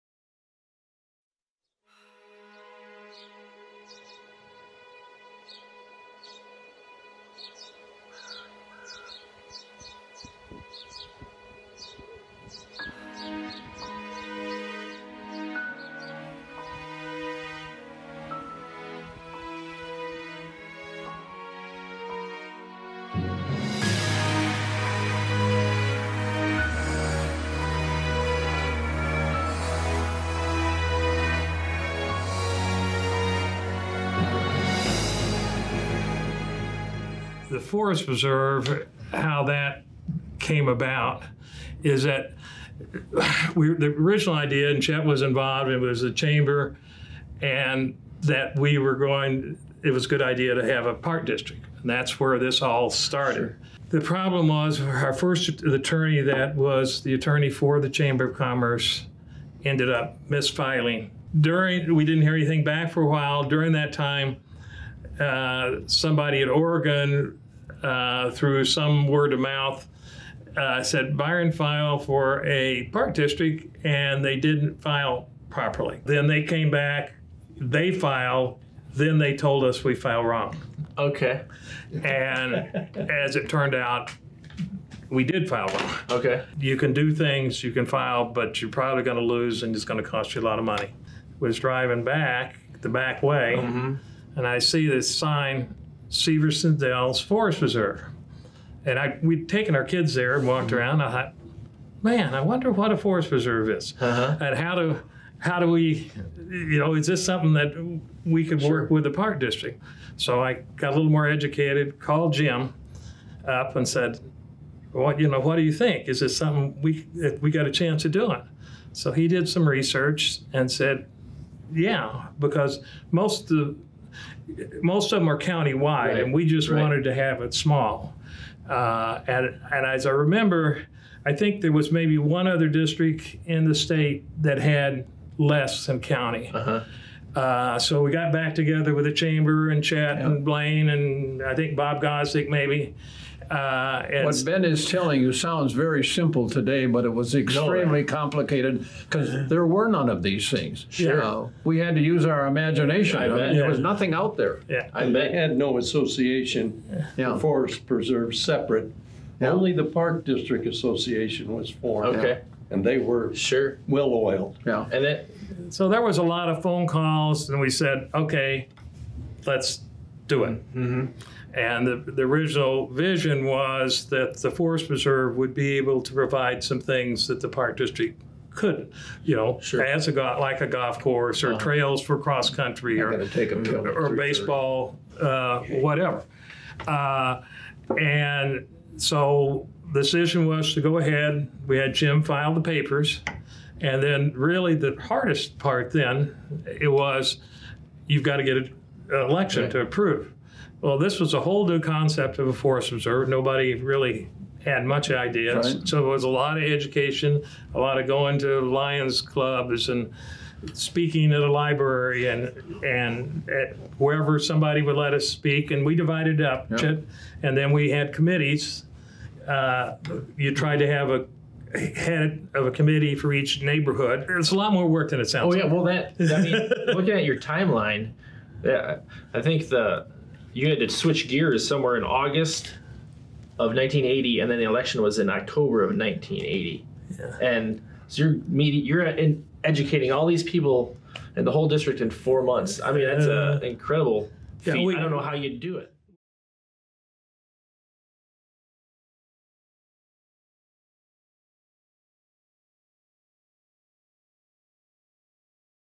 The Beginning of the Byron Forest Preserve District-Short Version
Short Version-
Two of the founding fathers and the first Executive Director sit down the the current (2020) Board President to tell the story of how the Byron Forest Preserve was founded.  They also share stories about the very early years in the District.

Ben Barton
Chet Kobel
Jack Philbrick
Trevor Hogan